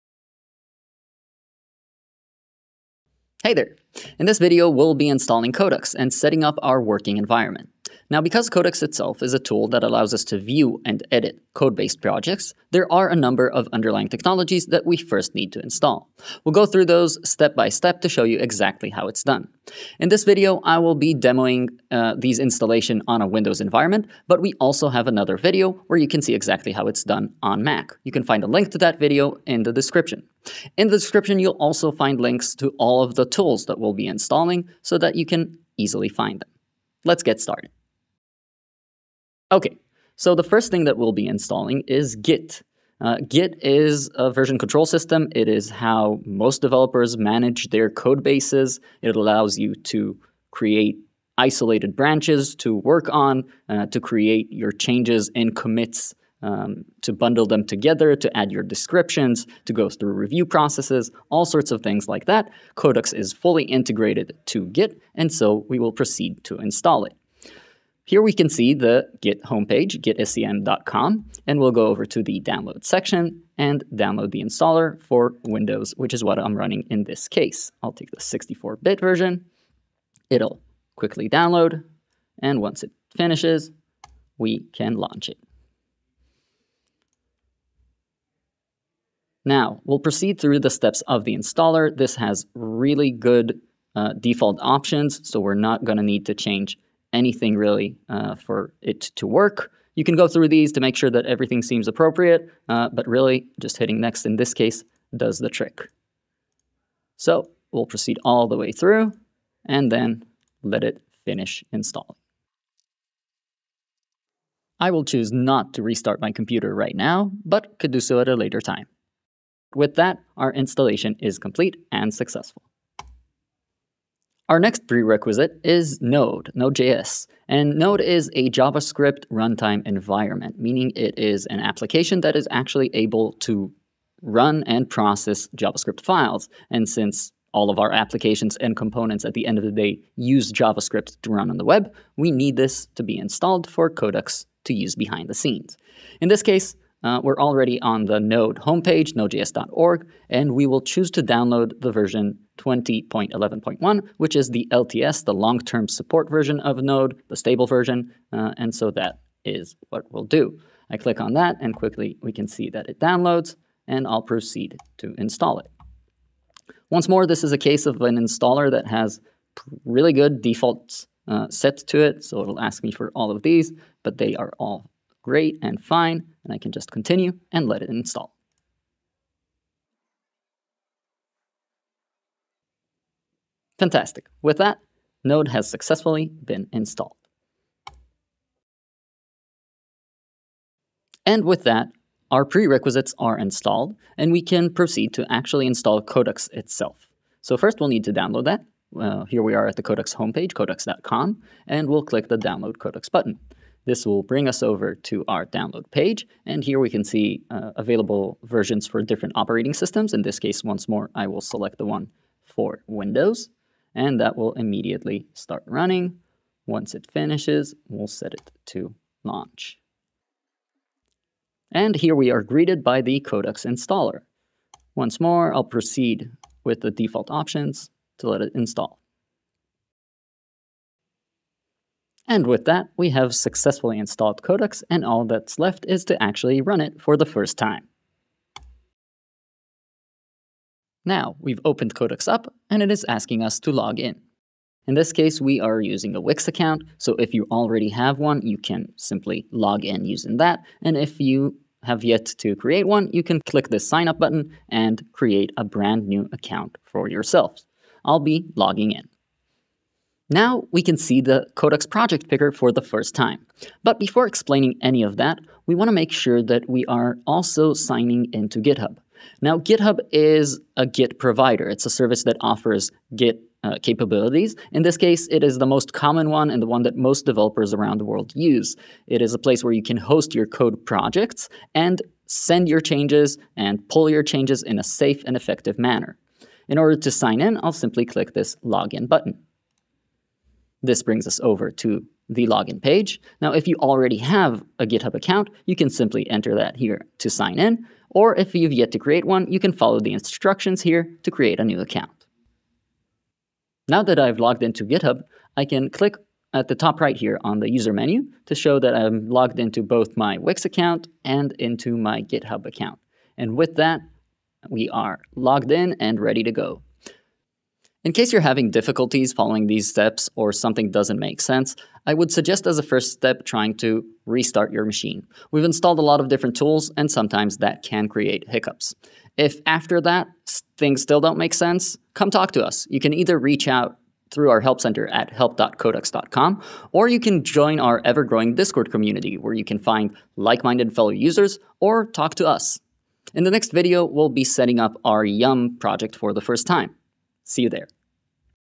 Installing Codux (part 2 of 6) | Codux 101 for Designers
In this video, we'll guide you through the essential steps of getting Codux up and running on your system. From downloading Codux to installing all the necessary prerequisites like Node.js and Git we've got you covered. Follow along as we lay down the foundation for your Codux journey.

Chapters:
00:00 - Intro
00:38 - Installing Git
02:03 - Installing NodeJS
03:08 - Installing Codux
04:20 - Connecting a GitHub account
05:18 - Next up